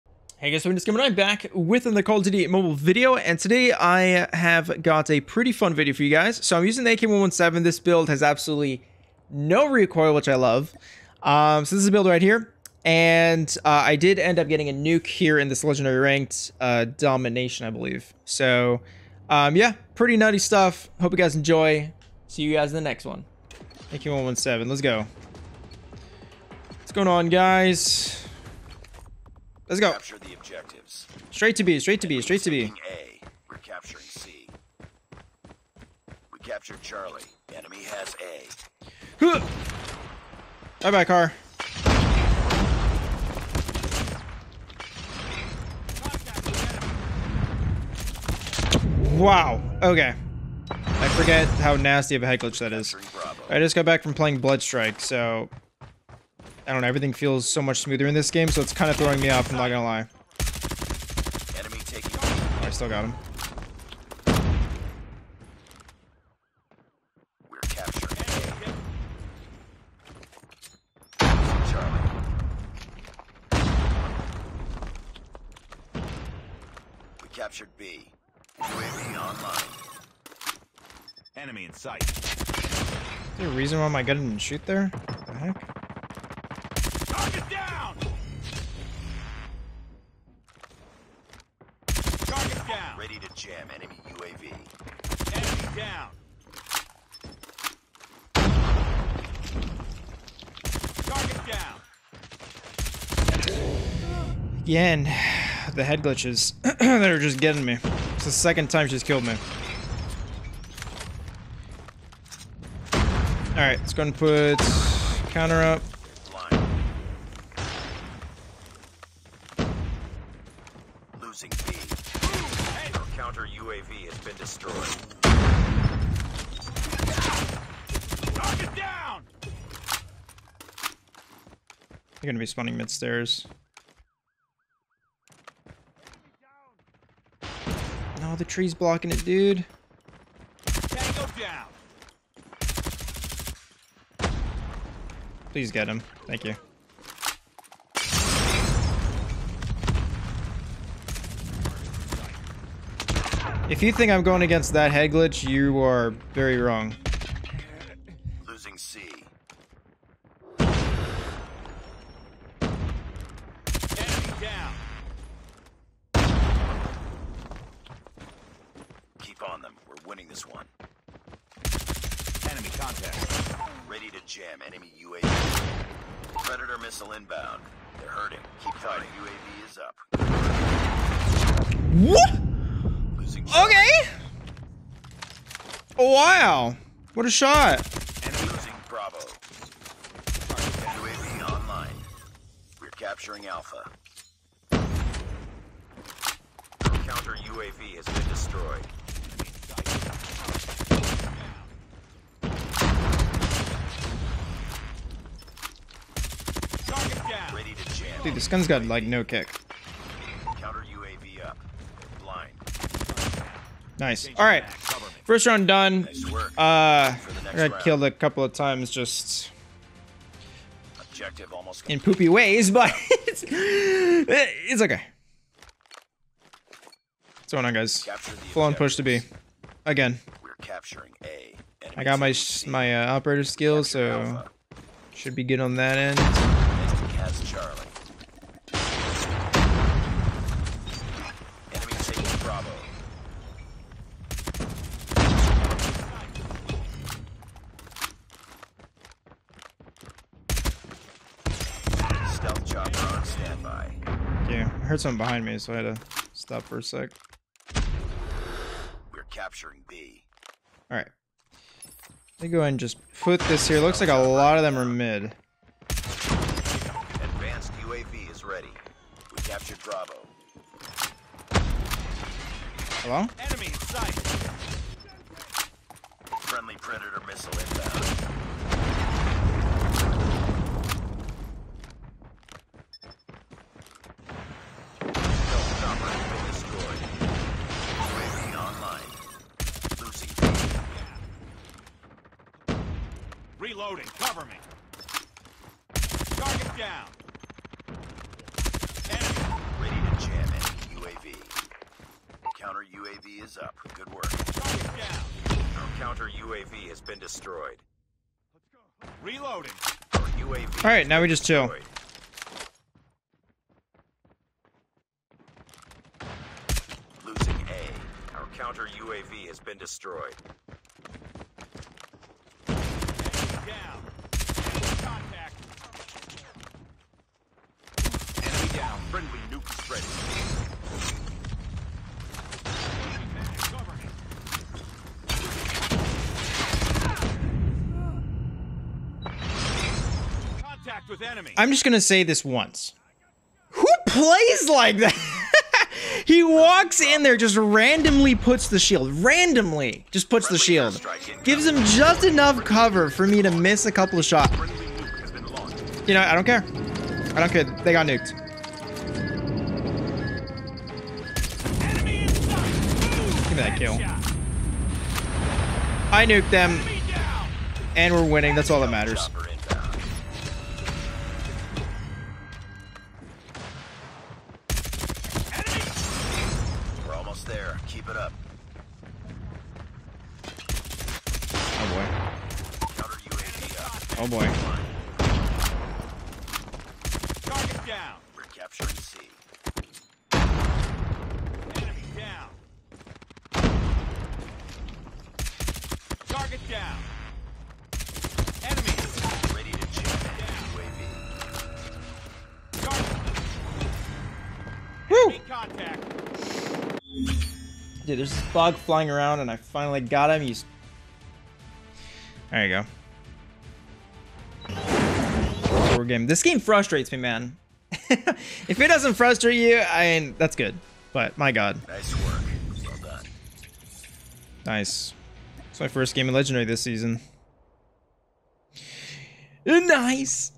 49 KILLS with this AK117 BUILD in LEGENDARY RANKED! (NUKE)
AK117 RANKED NUKE!